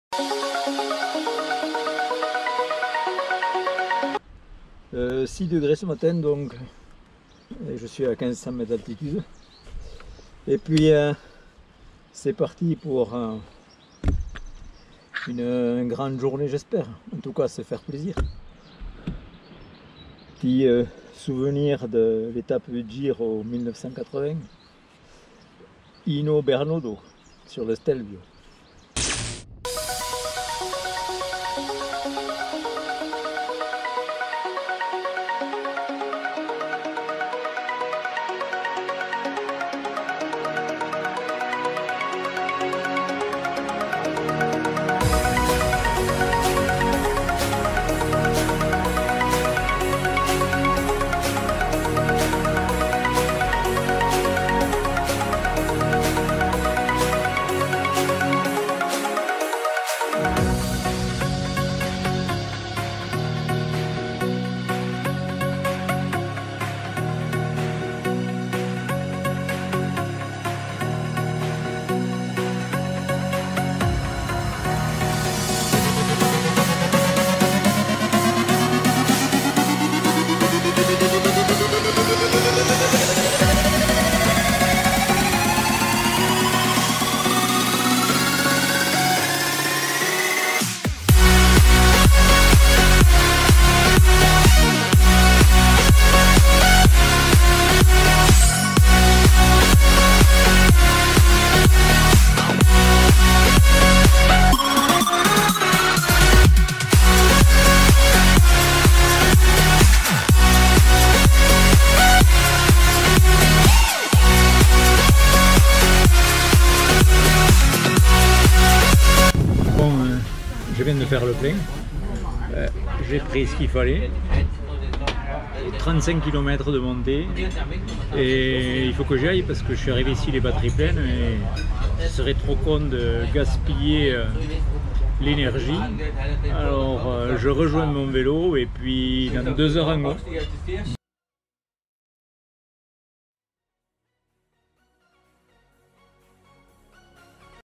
2' 30 au guidon de "TiltDragonFly" du Tyrol au pied du Stelvio
Le champion 2015 est indéniablement au rendez-vous de cette édition 2021. Certes il est surclassé par les deux vélomobiles de tête, mais son allure est digne des succès passés. Certainement aurait-il espérer un parcours plus montagneux et cabossé pour ce Sun Trip Europe. En attendant il a pris soin de réussir son étape du Stelvio et de marquer les esprits avec un temps d'ascension stratosphérique (au niveau d'un coureur cycliste professionnel).